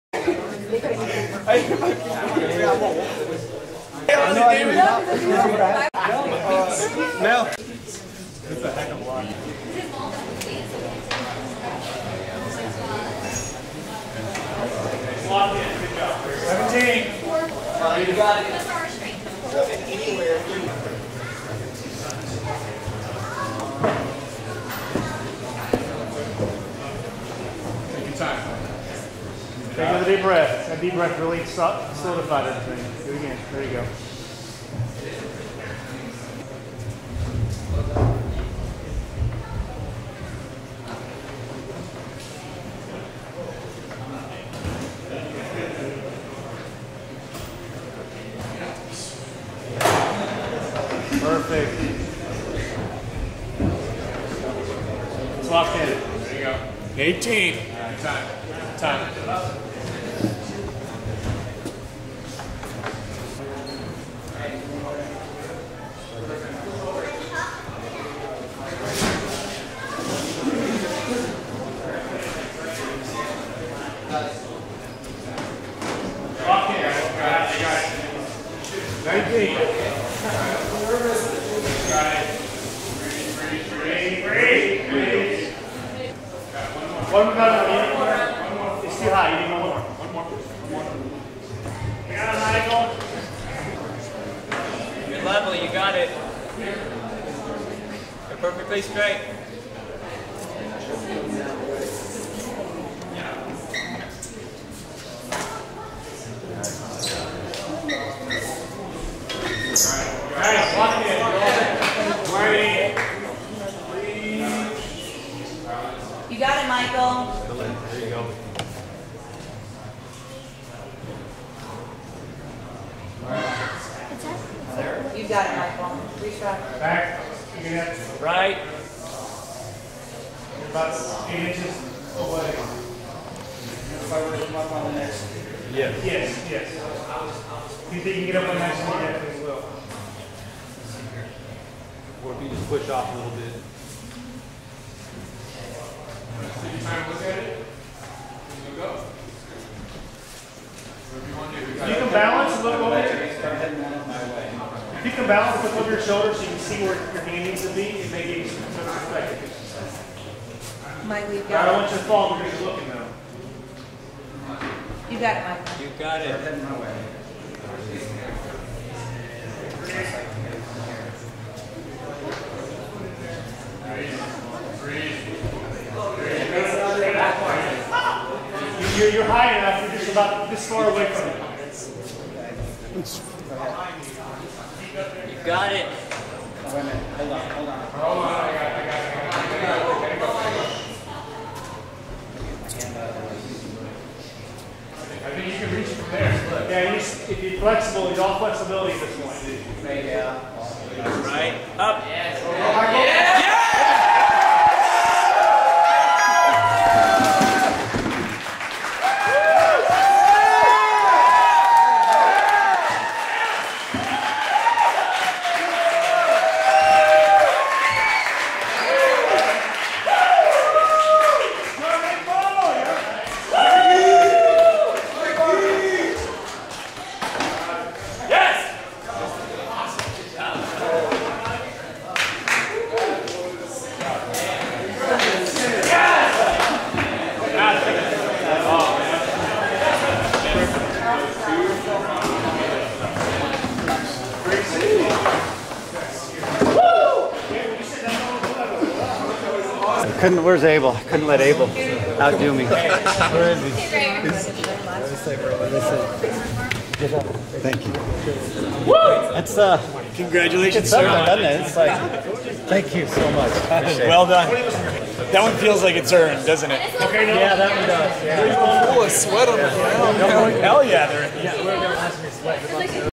Iron Ninja Mike stacking crates 16
American Ninja Warrior Mike is attempting to stack milk crates up so high that he can snag a pink ribbon off the gym ceiling at Iron sports in Texas